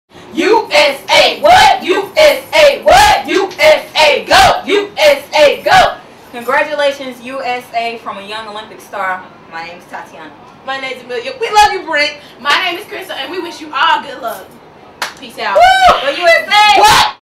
Charlotte Middle School is cheering for U. S. A.
Charlotte Middle School is cheering from Charlotte, NC